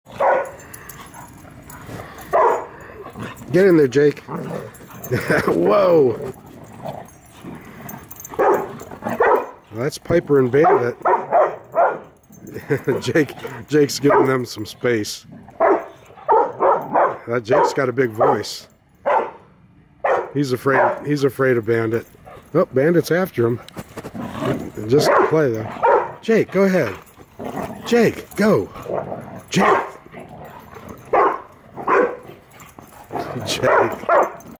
Stand-offish
Band, Marley and Piper playing. Jake has a very healthy "respect" for Bandit and wants to, but won't engage.